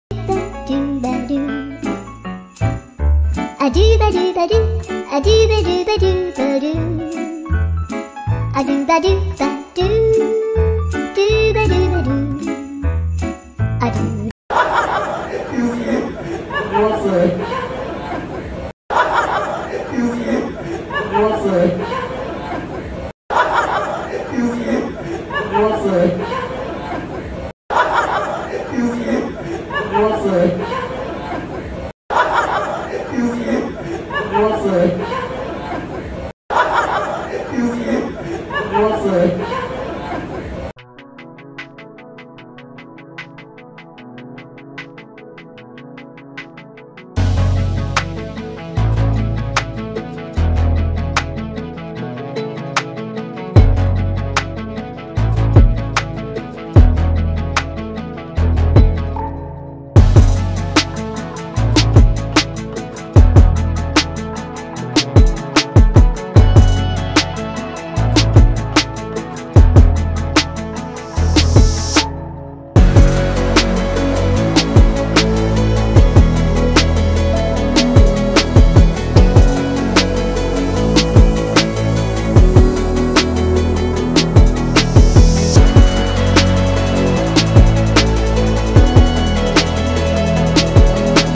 Allu Arjun Hilarious Fun With Ram Charan At Upasana Baby Shower Function | Telugu Cinema Brother
Allu Arjun Hilarious Fun With Ram Charan At Upasana Baby Shower Function